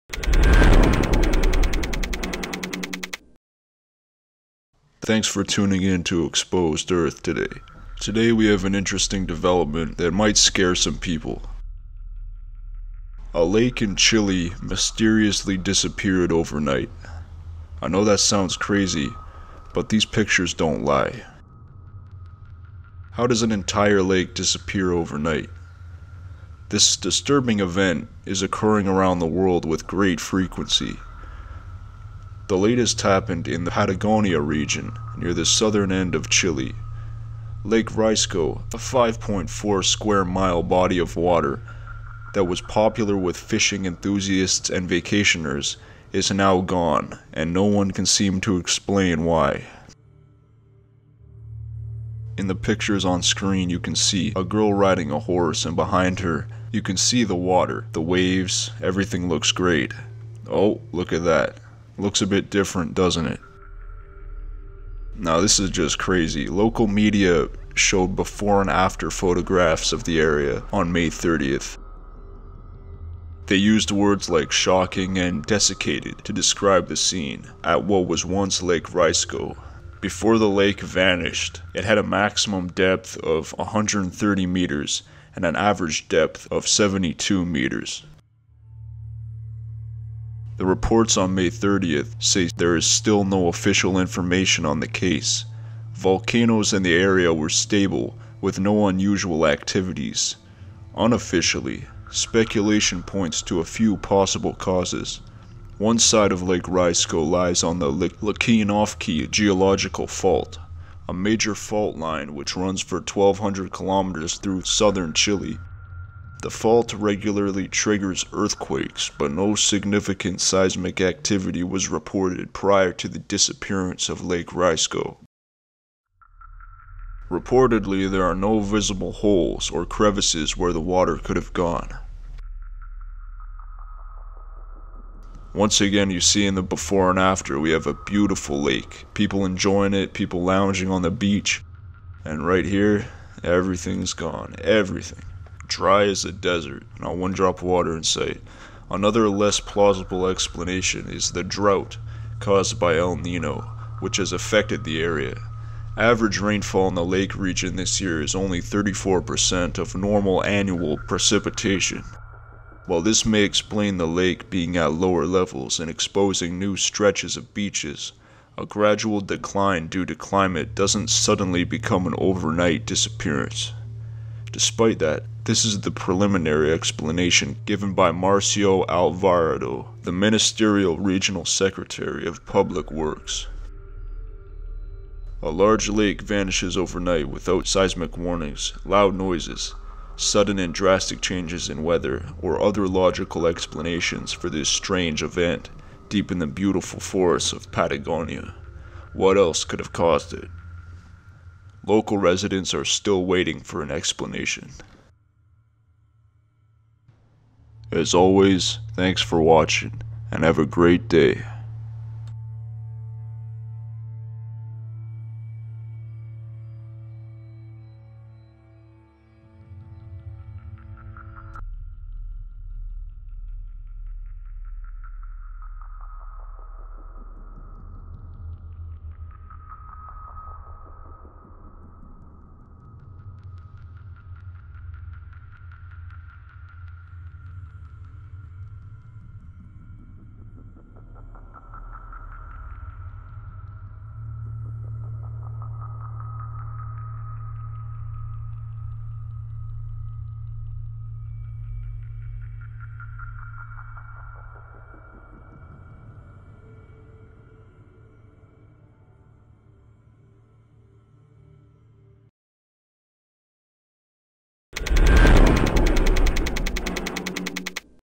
Lake Mysteriously Disappears Overnight
How does an entire lake disappear overnight? This disturbing event is occurring around the world with greater frequency. The latest happened in the Patagonia region near the southern end of Chile. Lake Riesco, a 1400 hectare (14 square km/5.4 square mile) body of water popular with fishing enthusiasts and vacationers, is gone and no one can explain why.